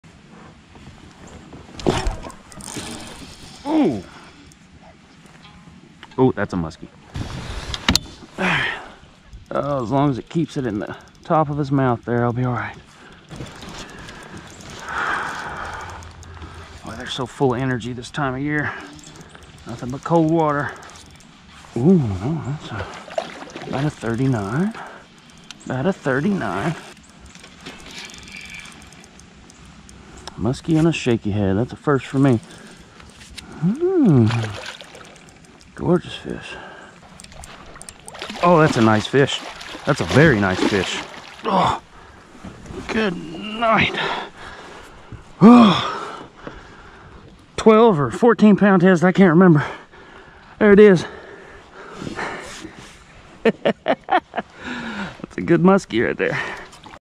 FAT MUSKIE (BASS GEAR) Kayak In December
Shakey head fishing for bass in my local river is a key way to catch some big fish in december! But there is always the chance of catching a muskie, and on this day, that exact thing happened! Check it out!